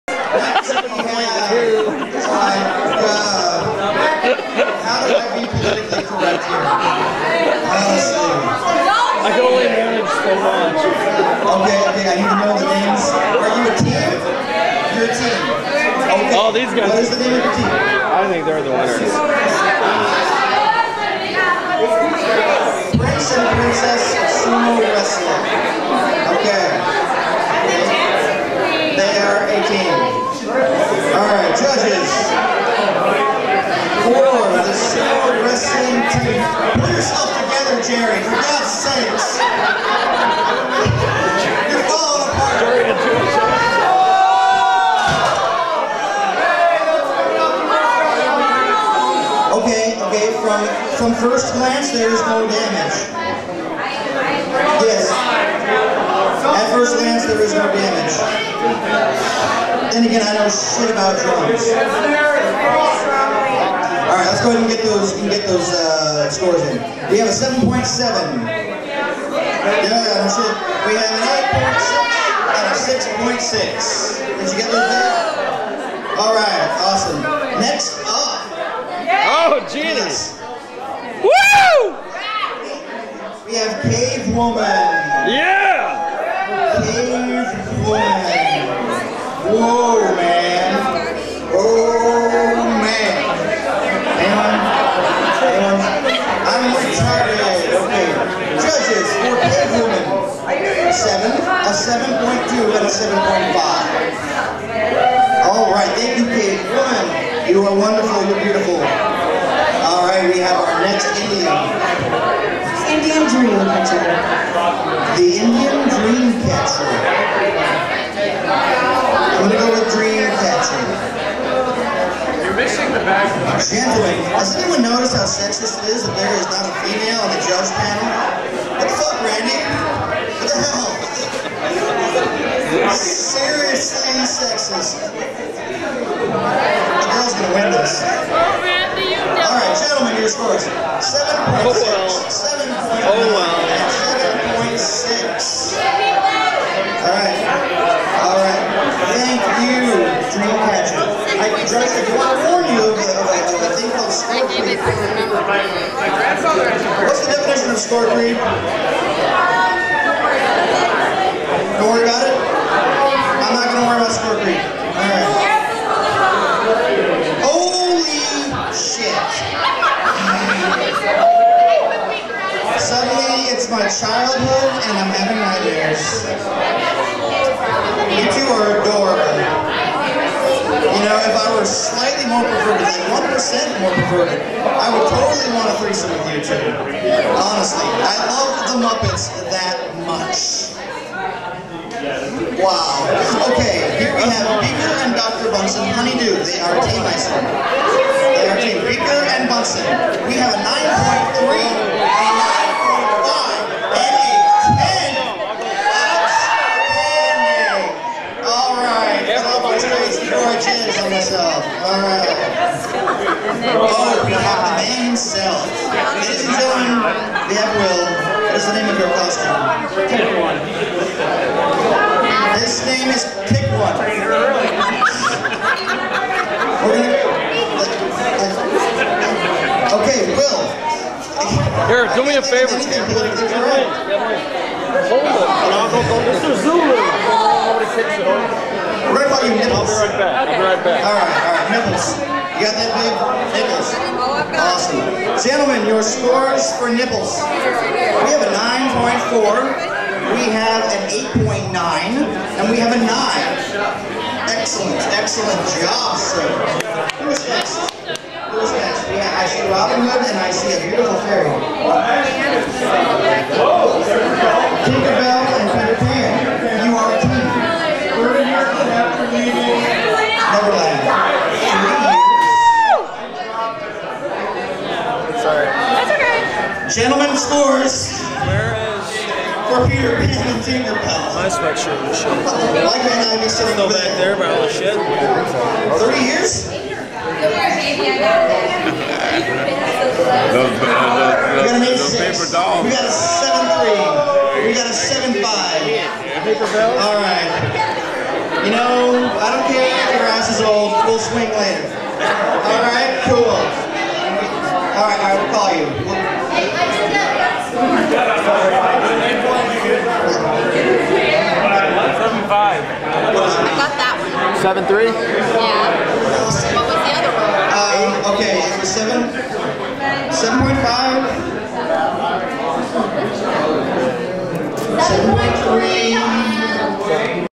Mosquitoe Halloween 10.30 The Pageant Continues
More costumed madness as Scott Crain presents the contestants to the judges. Celebrity judges include Weird Grandpa (Cam Harju), artist Robert Snell and artist Jerry Newbrough.  In this line up we have Team Prince and Princess Sumo Wrestlers, Cavewoman, The Indian Dream Catcher, Beaker and Dr. Bunsen Honeydew, Nipples, Peter Pan and Tinker Bell. Held in Kenosha, WI, the winner received a $25 gift card from Best Buy. Runner up received a $10 gift card.